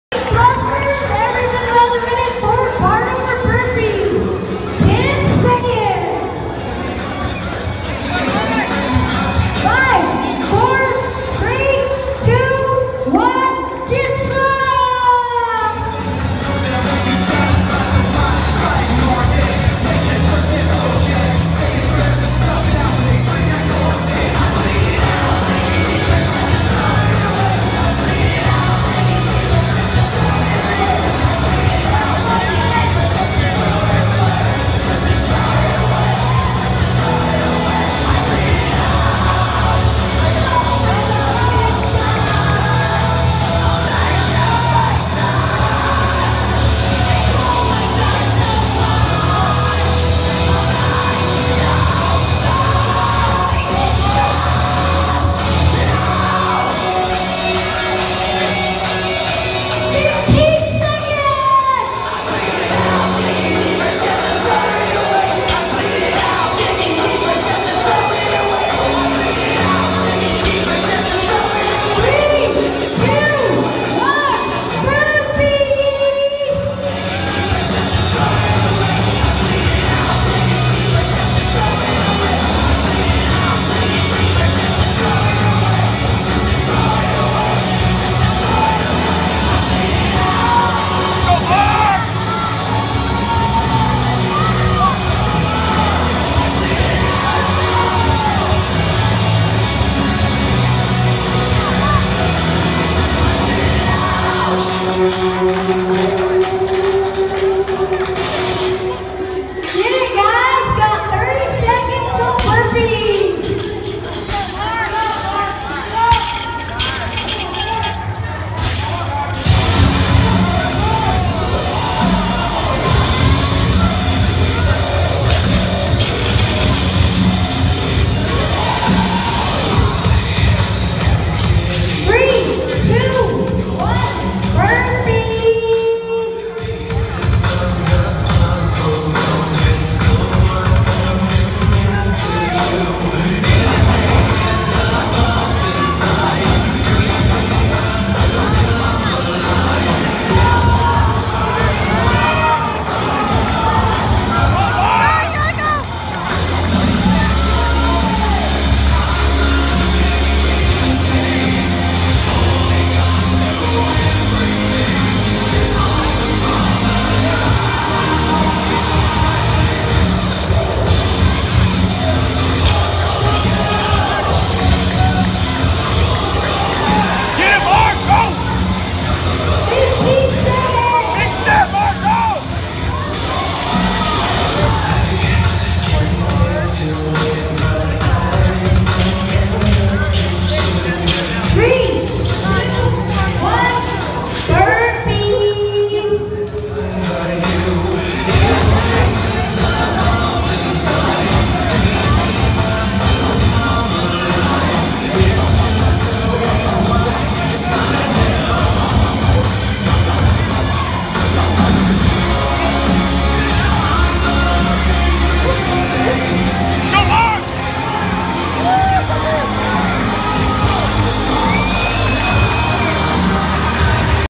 railyard games 17- Final
50 thrusters for time @135 (4 burpee otm)